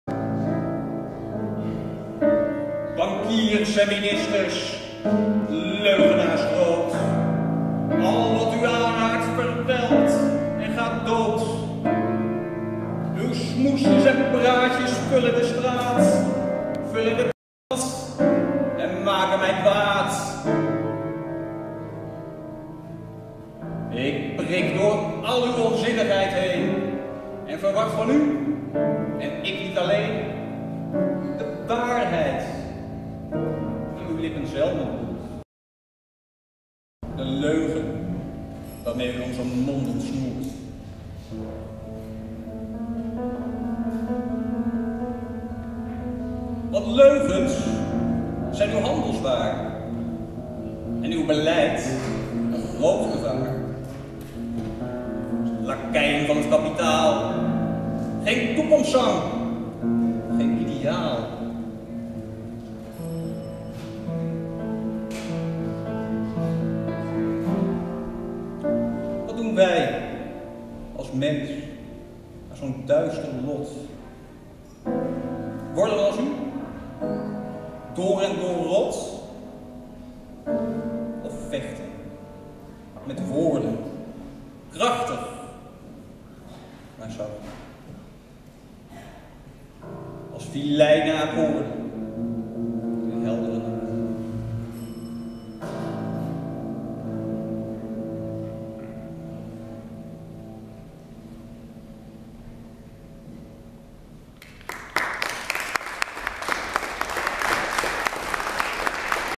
Het gedicht 'Politiek' tijdens het woord in Ruigoord
Patrick Bakkenes draagt het gedicht 'Politiek'  voor tijdens Het Woord in Ruigoord (19 april 2015).  Met muzikale begeleiding door pianist en meesterimprovisator Freek Zwanenberg